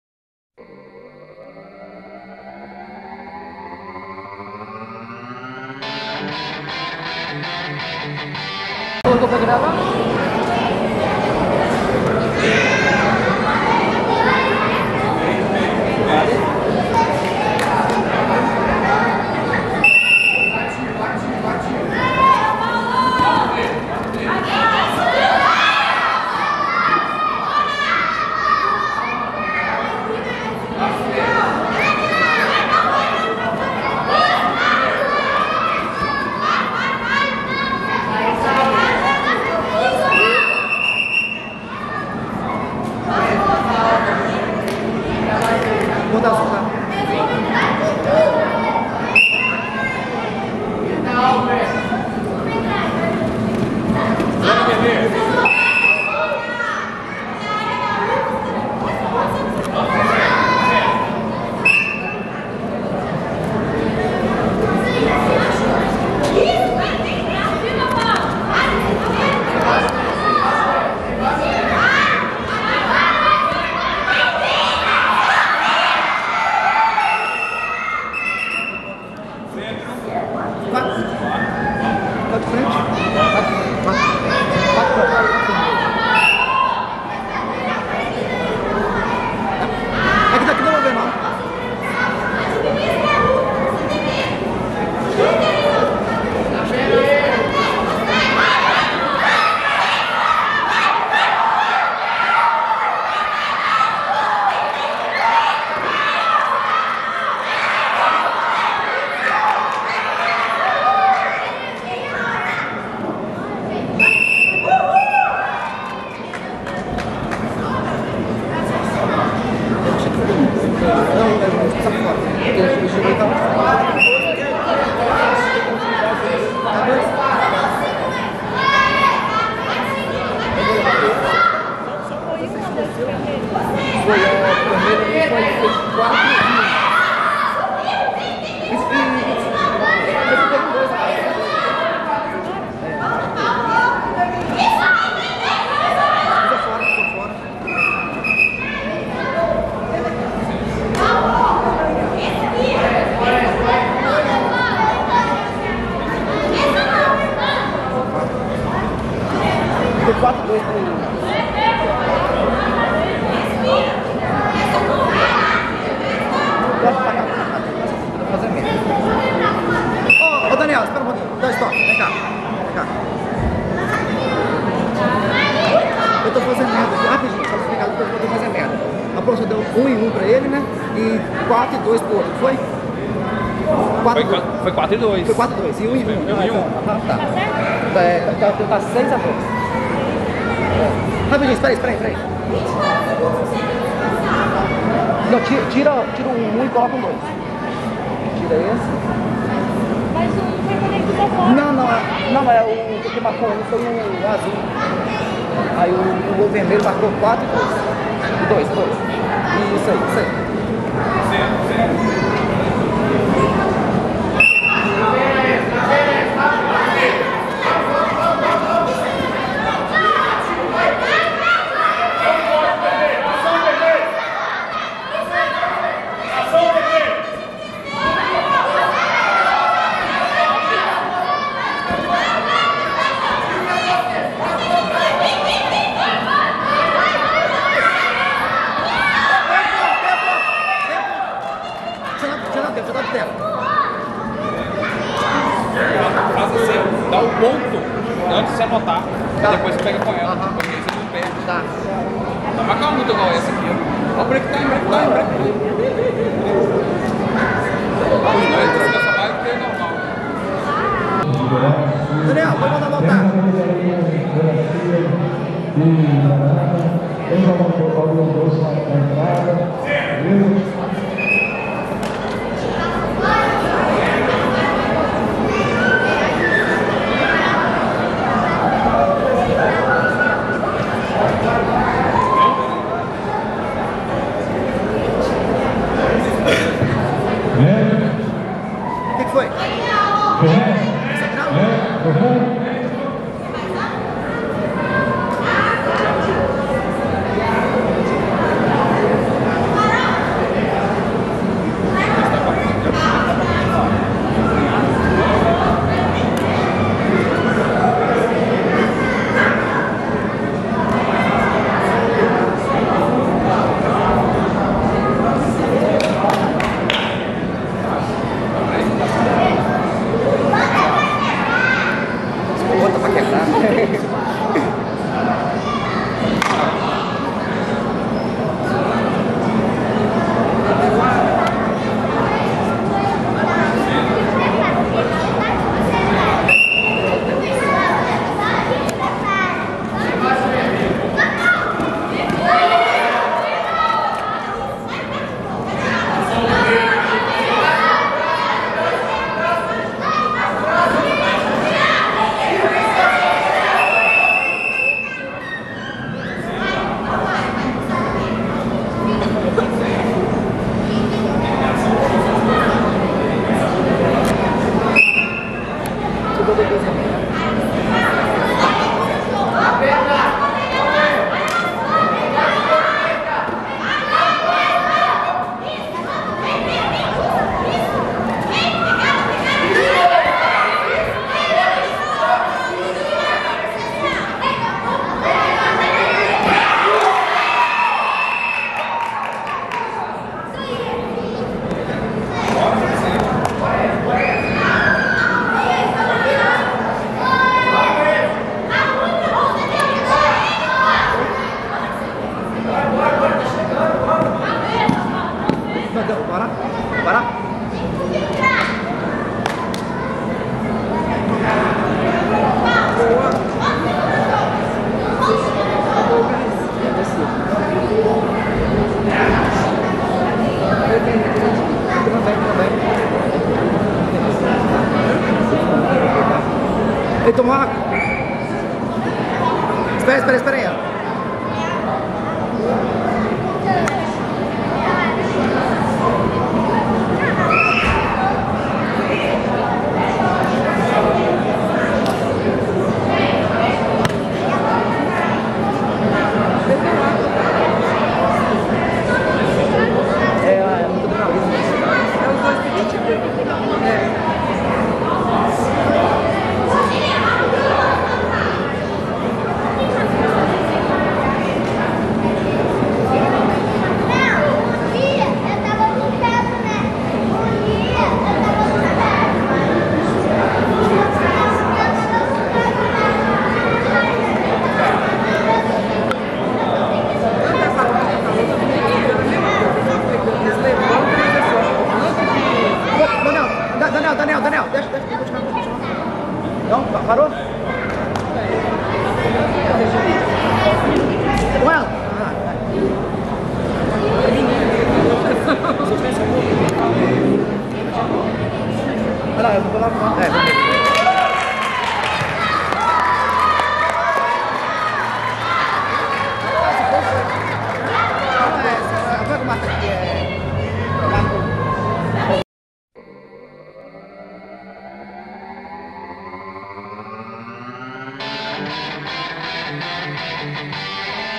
Paulo vs Enzo - Open Kids Wrestling 2
Combate de Wrestling entre Paulo Henrique e Enzo Santos no 2º Open Kids de Wrestling.